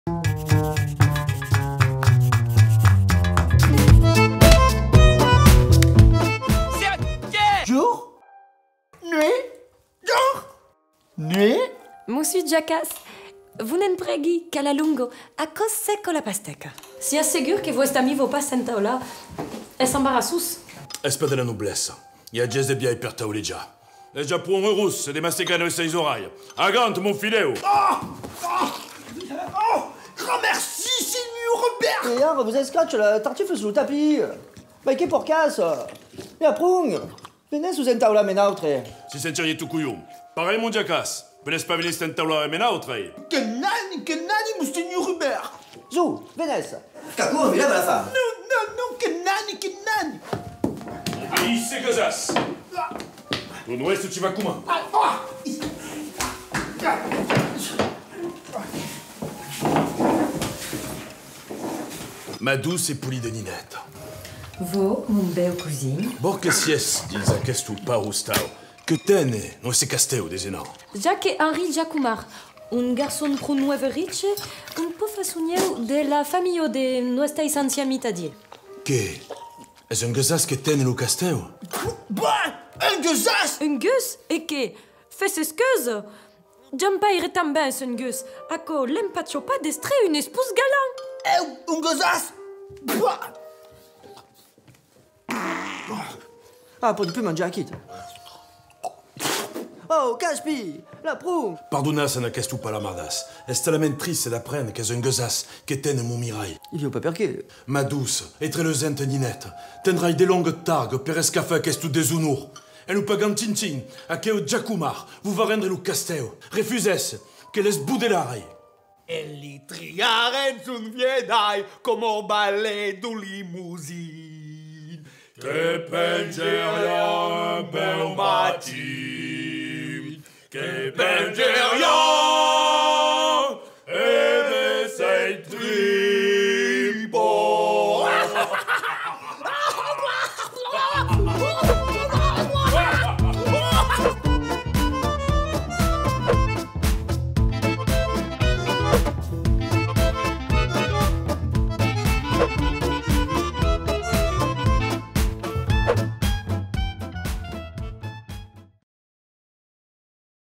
LEI VISIT'AIL-RE | "Les visiteurs" | ZÓU...M #5
Un vrai jobastre clique pour dérouler la barre d’infos ! ☀️

Découvrez notre série 100% provençale & 100% jobastres  👉 ZÓU...M 🎬

Voici notre premier épisode de cette série ZÓU...M “Lei visit’ail-re”

On espère que cela vous aura plu, on se retrouve bientôt pour un nouvel épisode !

Voici le nouveau crédo  “laissez-nous un like, un commentaire ou partagez la vidéo si ça vous a plus” et GRAMACI  ;)

Montmirail - Jean Michel Turc
Jacquouille - Romain Borelli
Jean-Pierre - Nicolas Amat
Béatrice - Oriane Faure

Écriture - Les Jobastres & Jean-Michel Turc
Production - Les jobastres
Traduit - Jean-Michel Turc

Réalisation - Hugo Balique
Montage - Hugo Balique
Chef opérateur - Cyril Pedrazzini
Mixage et sound design - Samuel Poirée
Générique - Gabrielle Guerreiro
Assistants plateau - Etienne Cot & Cyril Innocente

Miniature - Laurie Assaud

D’ailleurs, t’y as compris ou t’essplique ?!